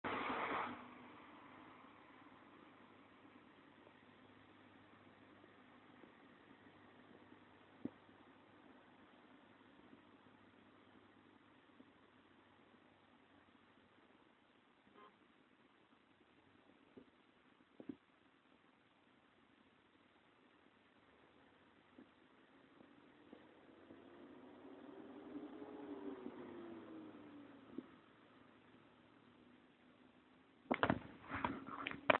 Радикальная уборка территории
Сценка из нормальной городской жизни. Убирают пыльную улицу.